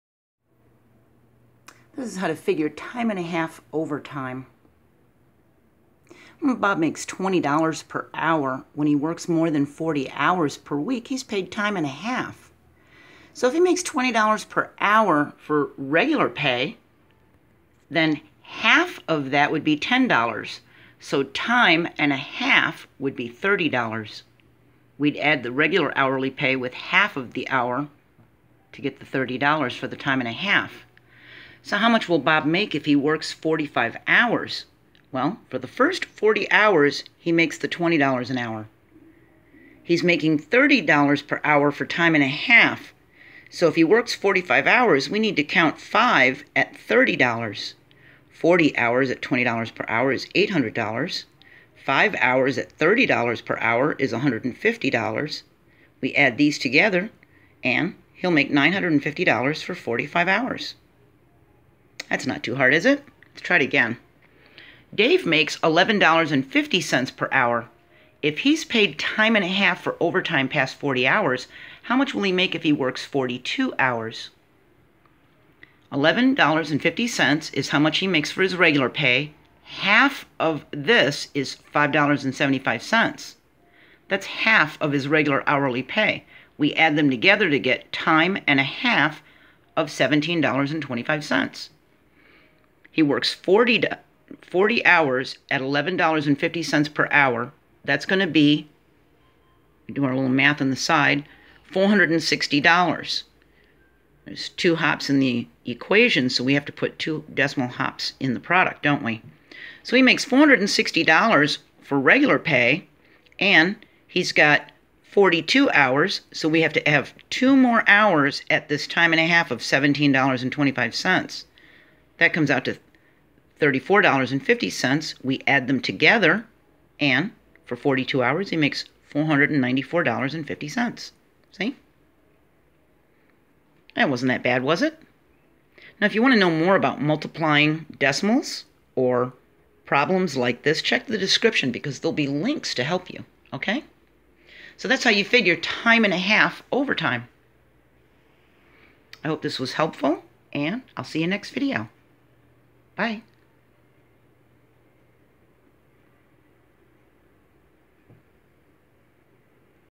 Figure out time and a half overtime
An explanation of how to find the amount of a paycheck if paid "time and a half" for overtime.